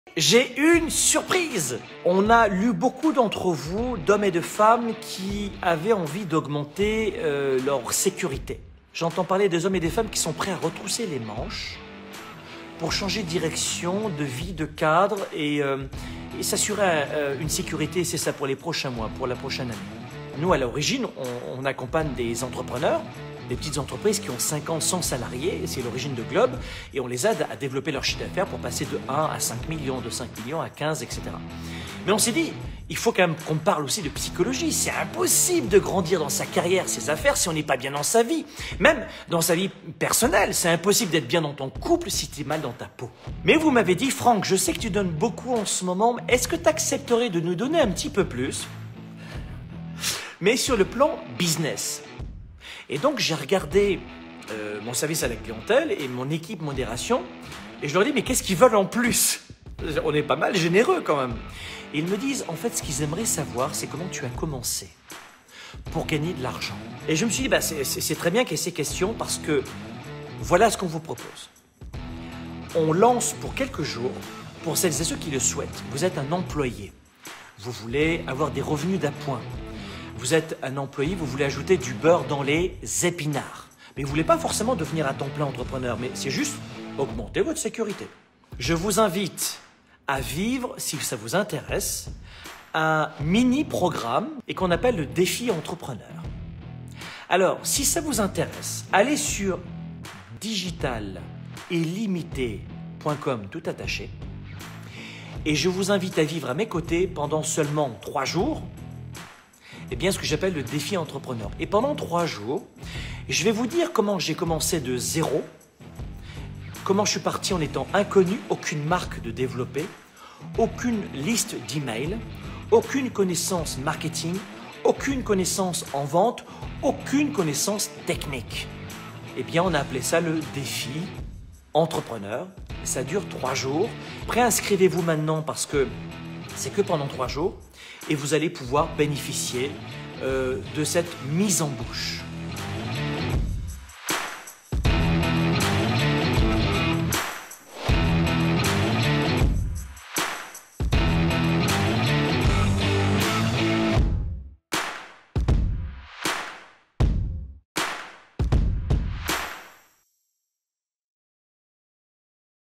Comment vivre de votre passion en toute liberté I Franck Nicolas
“Les choses rentrent dans l’ordre”

Pas du tout...

En Mars 2020, nous avons été privés de nos libertés.

Cette secousse mondiale a entraîné un profond changement de nos habitudes et de nos
modes de travail.

De cette pandémie découle une crise financière dont les impacts se feront ressentir durant
de nombreuses années.

Cependant, cette révolution de nos modes de vie entraîne des opportunités sans précédent.

Vous n’avez pas besoin de compétences en marketing ou en ventes.

Vous n’avez pas besoin d’avoir des followers.

Vous n’avez pas non plus besoin d’être un génie en informatique.

Ni d’avoir beaucoup d’argent...

De quoi avez-vous besoin?

D’être sérieux et d’avoir une réelle volonté de décupler vos revenus.

P.S. C’est gratuit!